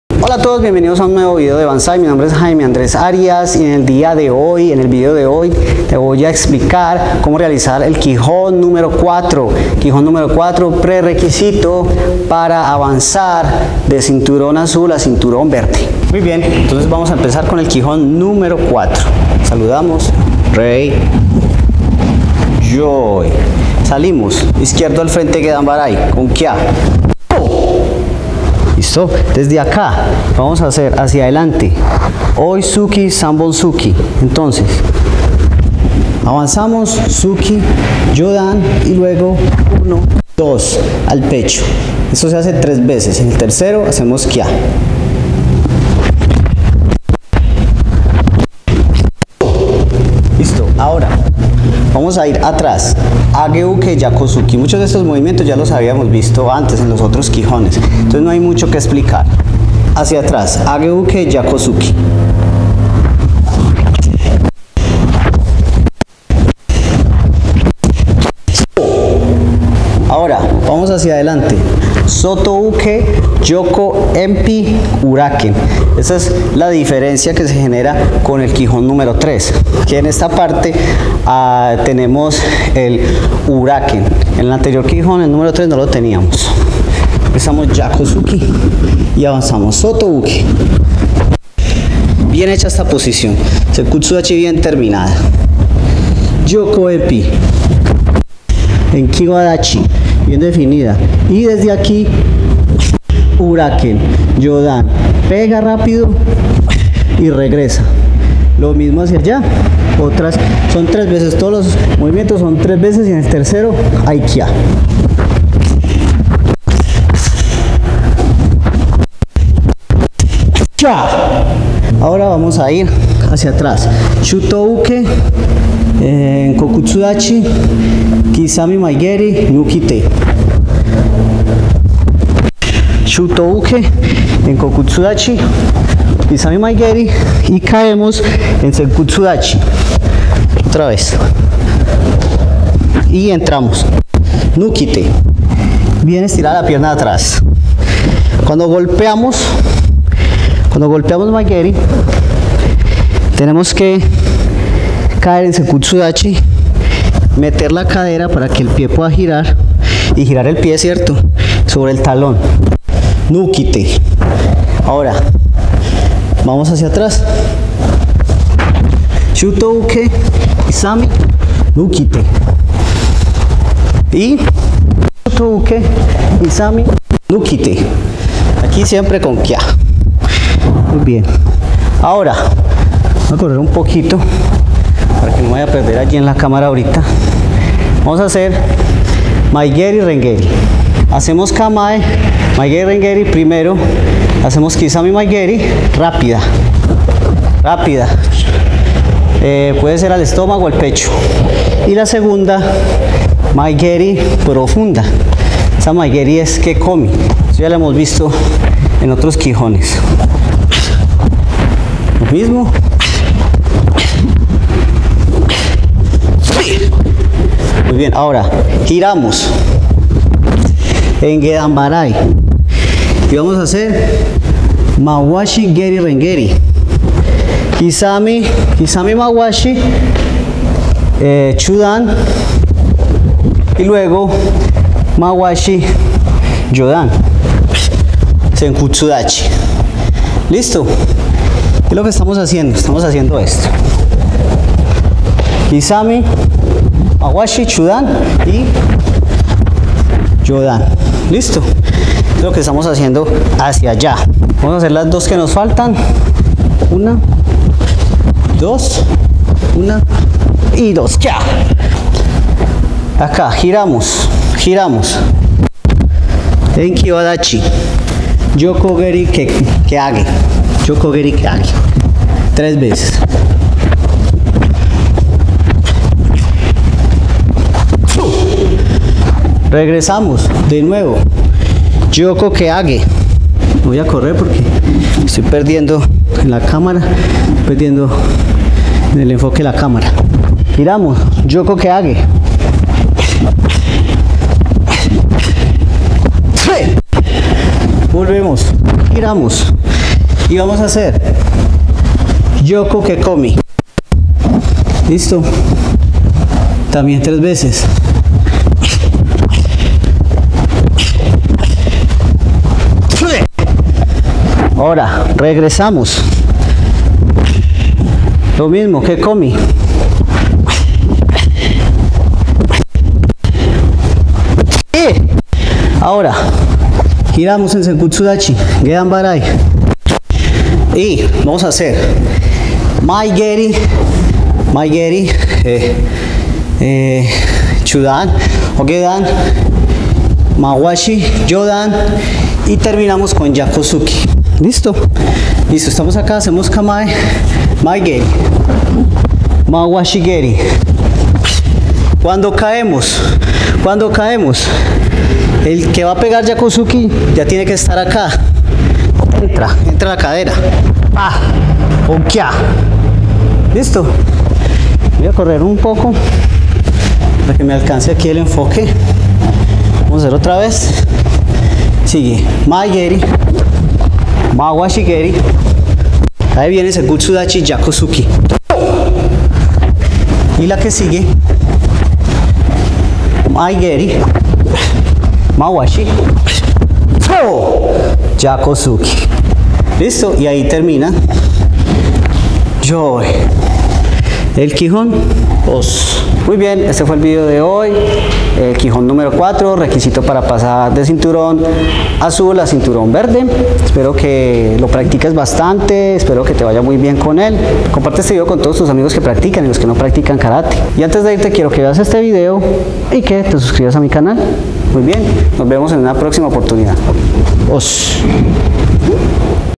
🥋 KIHON de KARATE 💥 SHOTOKAN 4
En este video aprenderás a realizar el kihon para el examen de cinturón verde (Las técnicas varían según la escuela).

💥¿QUIERES REALIZAR KATAS MÁS VELOCES Y CON MÁS POTENCIA?💥
Ingresa a mi curso: Entrenamiento Básico de Potencia para Kata

¿TIENES PREGUNTAS?